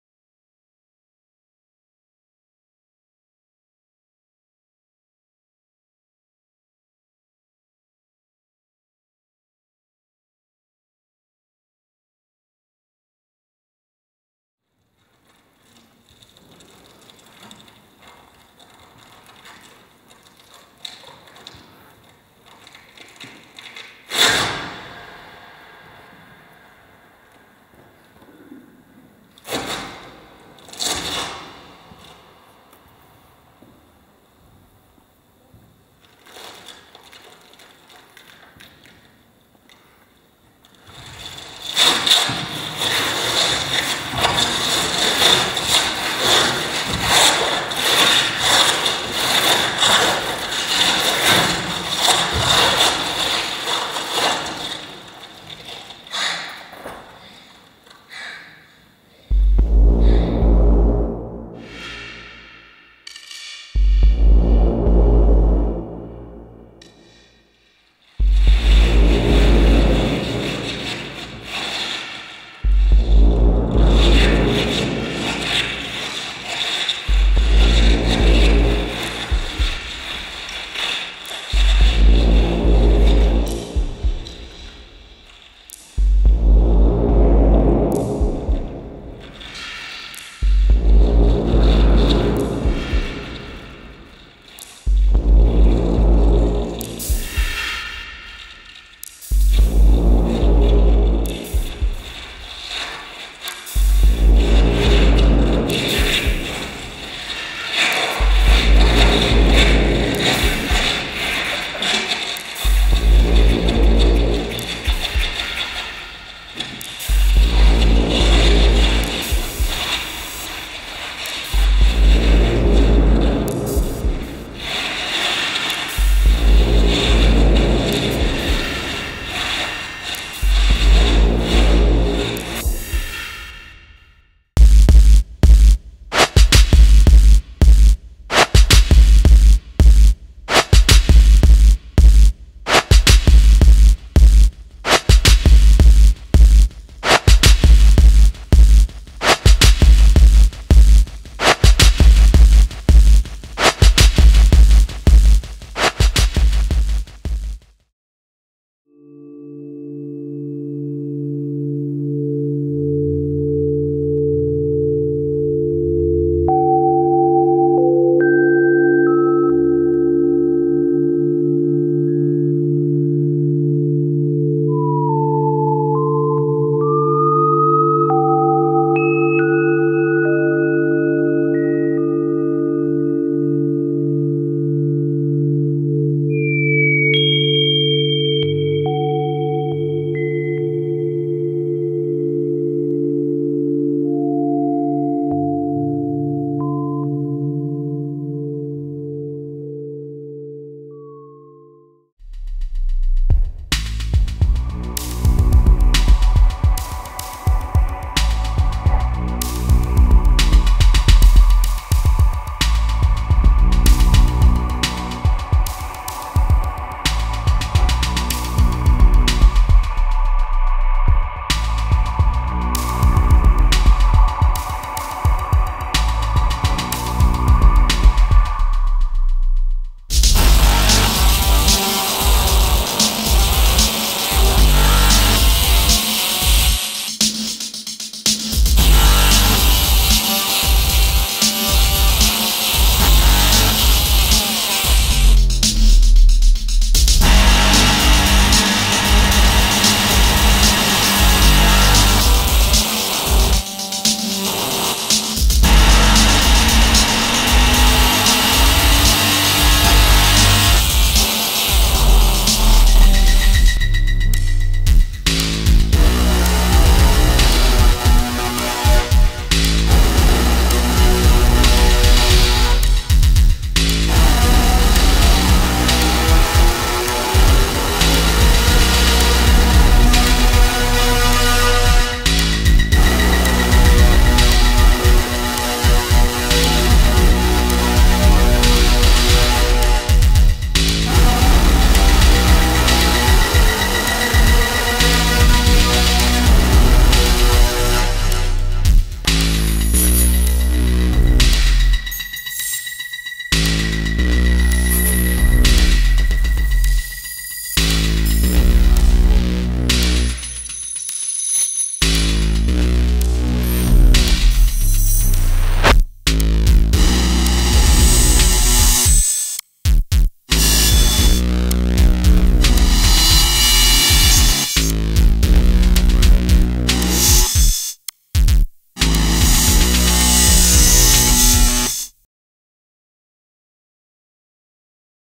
Gabriella Cerritelli - Mara's Attack 2008
Live at Cavallerizza, Torino, IT

Mara's Attack is an installation in which the body of the performer is an integral part of the work.  The research of performance and image is  focused on the close relationship among body, material, and space:  between the limit of the material and the expansion of being.

Installation/performance of art and dance
Gabriella Cerritelli concept/performance
installation Paolo Grassino
costumes Roberta Vacchetta
music advisor Gabriele Bramante
editing Andrea Daddy